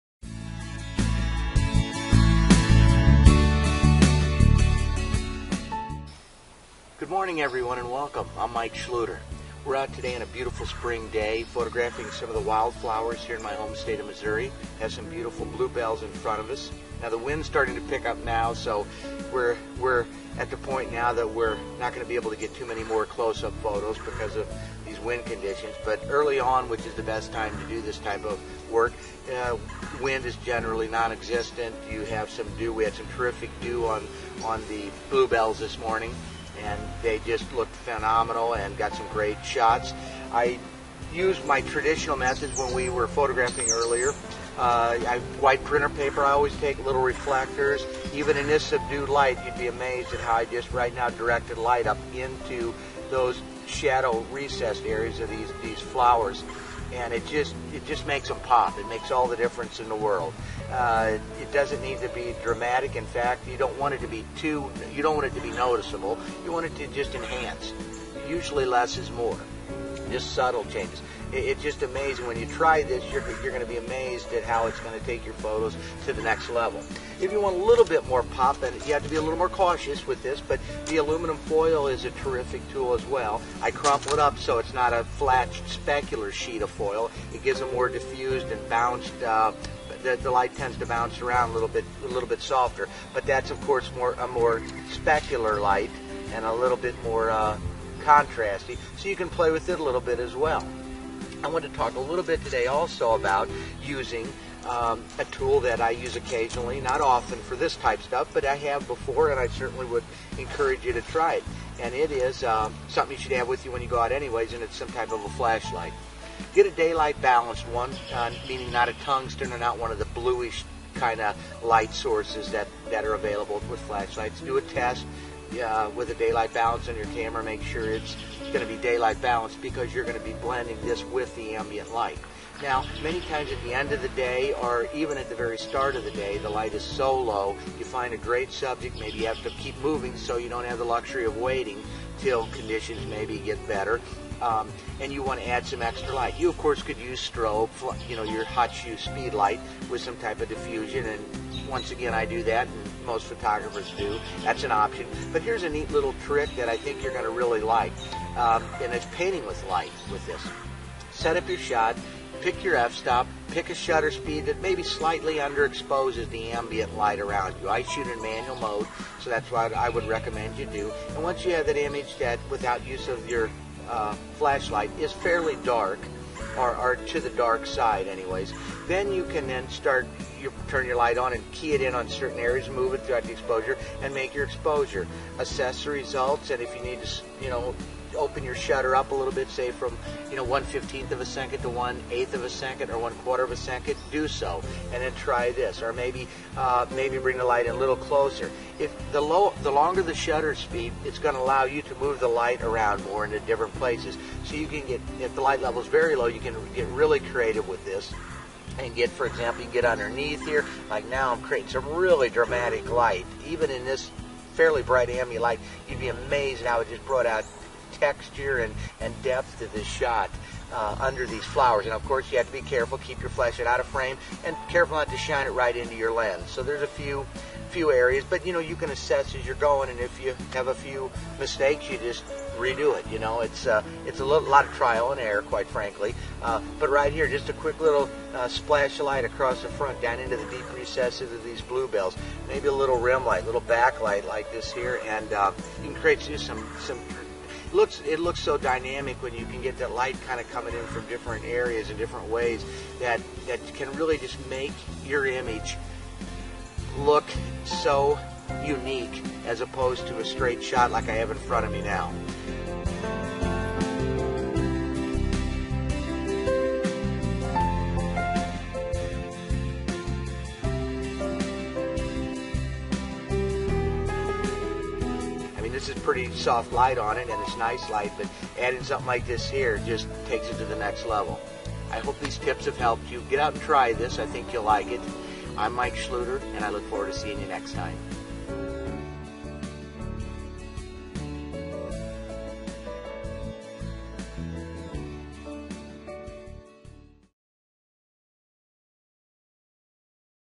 How to take photos of Wildflowers 2014 Ninja trick!
Michael Schlueter shows How-to create photos using light painting techniques, in his Free Videos Tutorials.
Ninja tricks! use a flash light to paint!